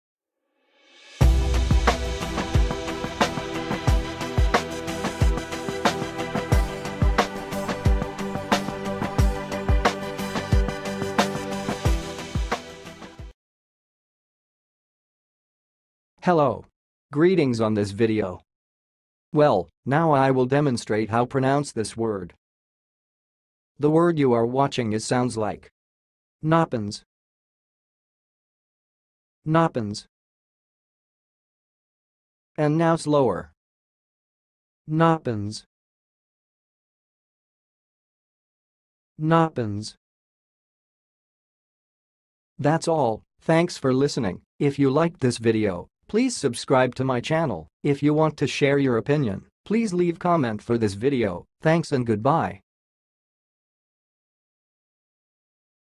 NOPPEN'S - HOW TO PRONOUNCE IT!?
Watch how to say and pronounce "noppen's"!